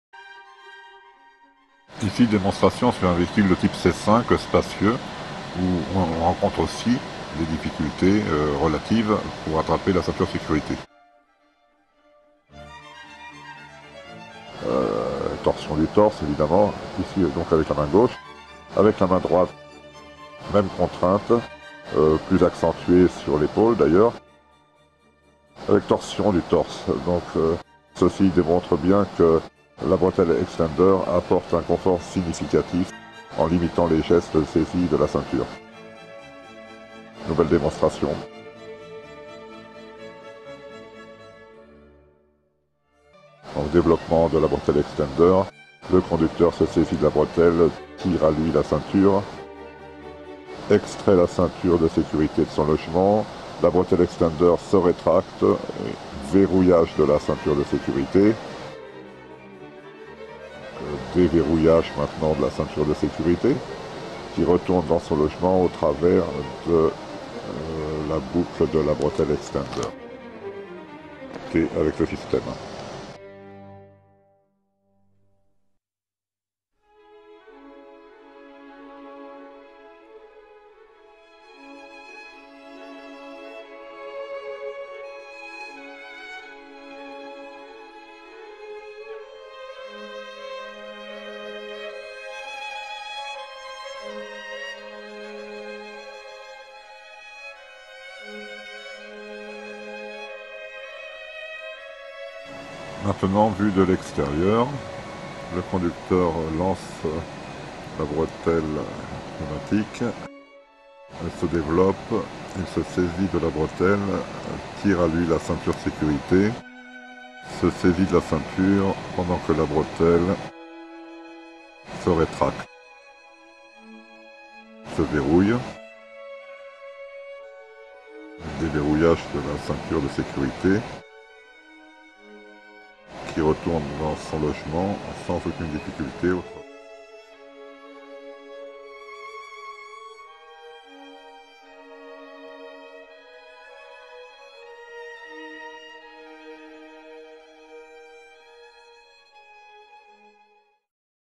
CEINTURE SECURITE CONFORT PNEUMATIQUE POUR CEINTURES DE SECURITE
BREVET FRANCE 2008 . S'INSTALLE EN QUELQUES MINUTES ( VERSION MOBILE) DANS LA PLUS PART DES AUTOMOBILES.REDUIT LES CONTORSIONS ET AMPLITUDES DES GESTES POUR SAISIR LA CEINTURE DE SECURITE DE 80% !!